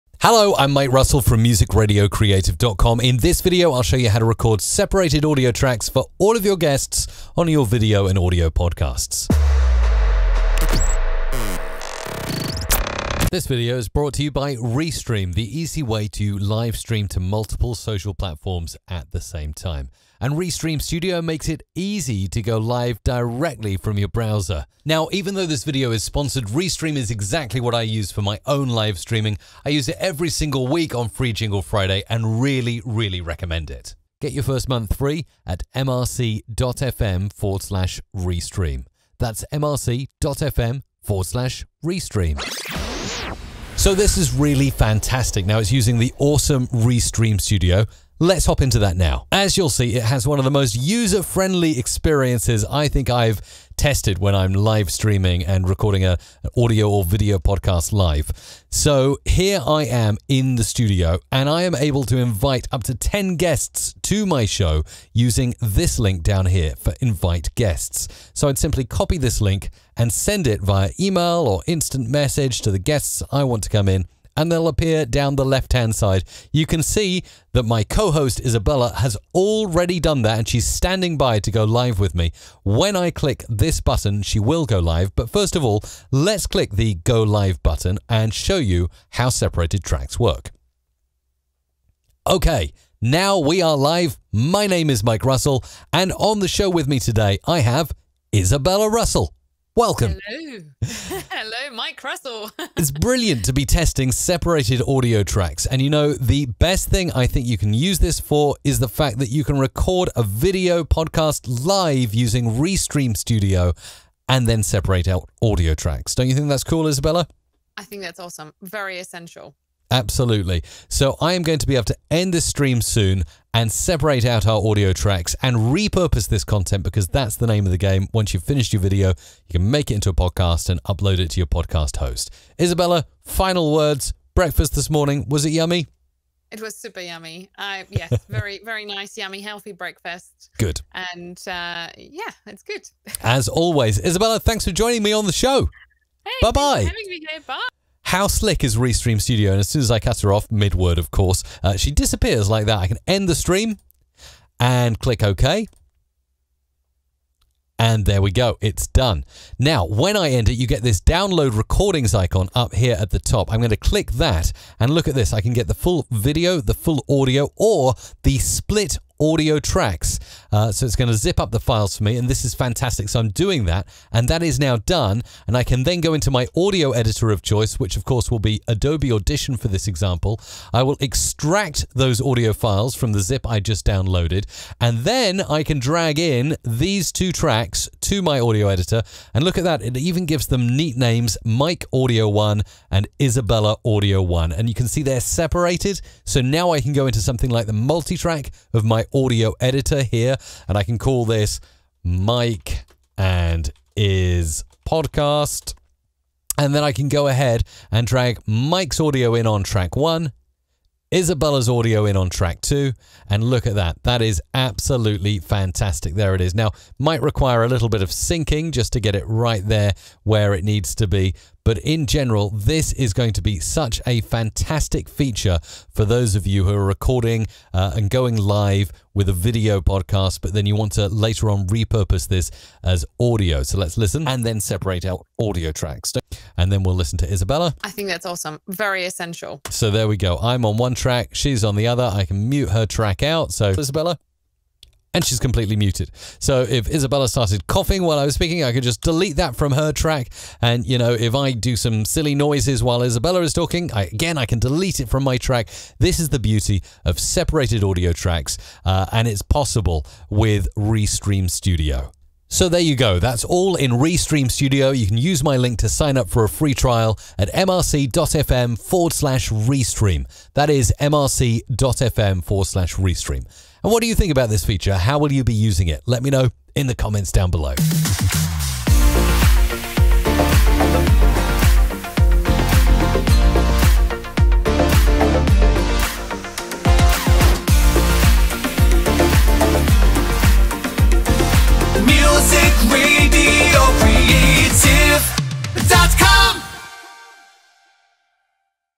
Unlock the Secret to High-Quality Live Streaming: Mastering Separate Audio Tracks in Restream Studio
Restream Studio allows you to record your live stream audio and split it into seperate tracks! This is ideal for repurposing your live video stream into an audio podcast.

0:00 Tutorial Start
0:43 Getting started
0:58 Invite guests
2:39 Download recordings
3:08 Importing to Audition
4:22 Wrap up